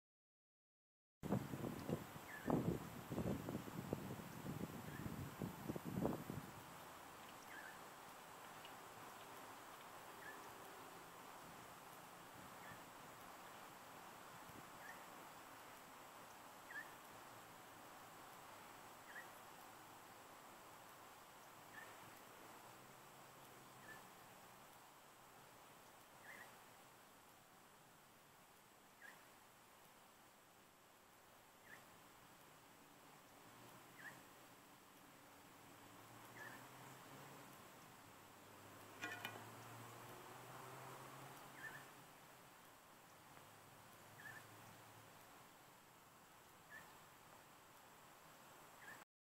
Redneck Bird feeder at work
Humming Bird Feeder Made from Lids works like a charm. Here is the proof.